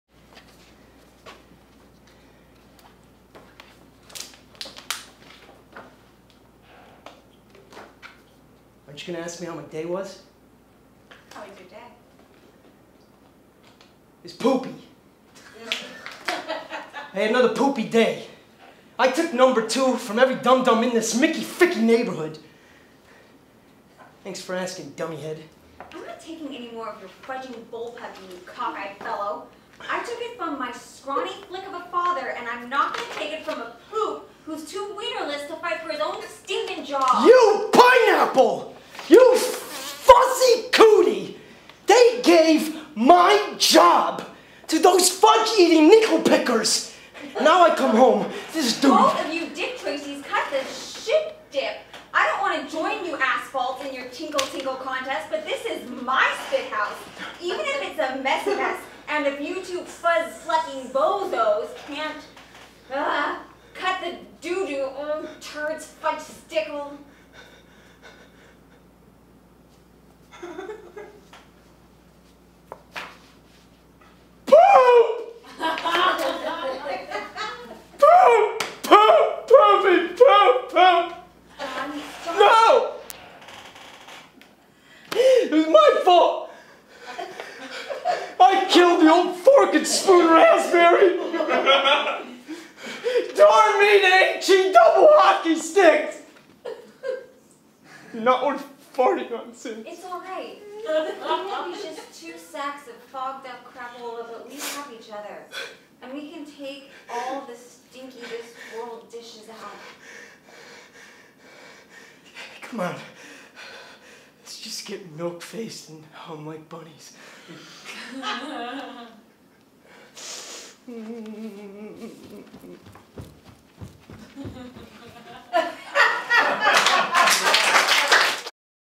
A scene from 'Tenement' by William McGuire (*Clean Version)
A fun reenactment from a scene I did in a class once.

It was originally made famous by legendary method actor Kenneth Marino of MTV's THE STATE.

A sketch intended to class things up on the show with a dramatic turn. Tough, they did have to soften the language a bit for television.